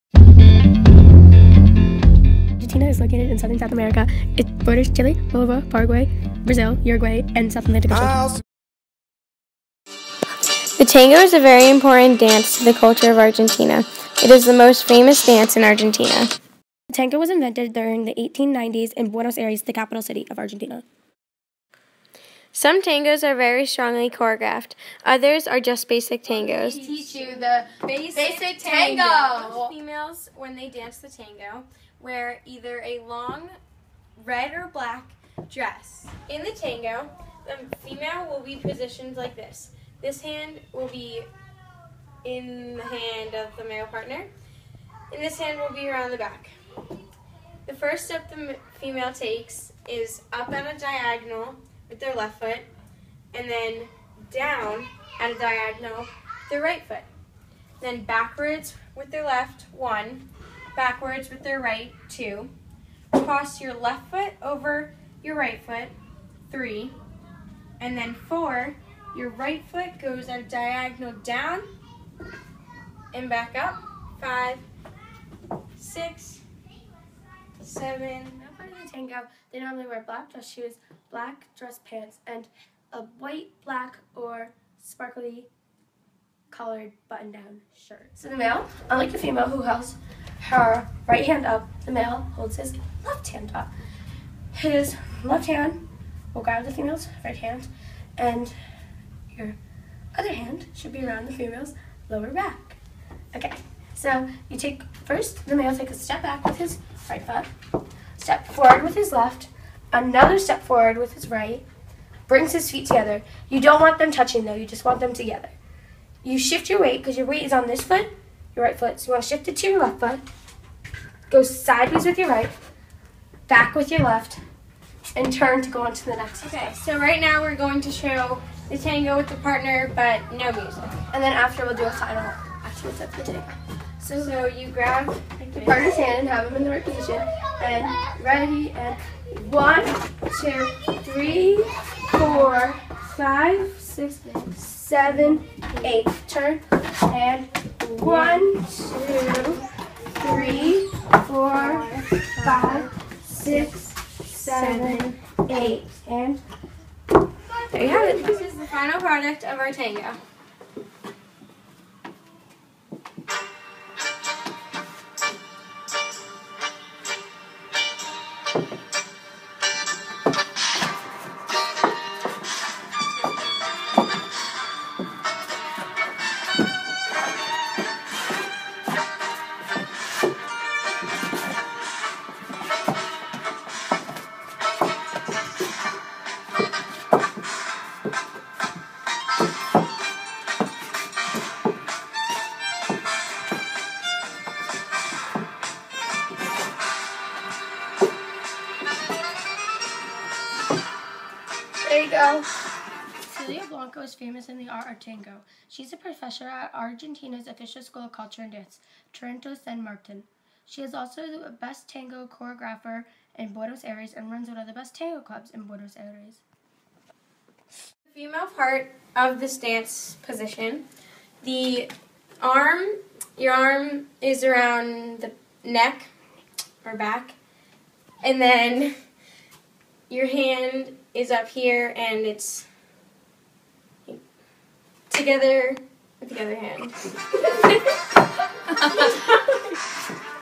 Argentina Tango
By Karolina S and Callie S

Each student group was responsible for creating a video or in class presentation about a cultural element of an assigned country. In the presentation, students should briefly explain the history of topic and area, need to know information, and if possible, a small demonstration.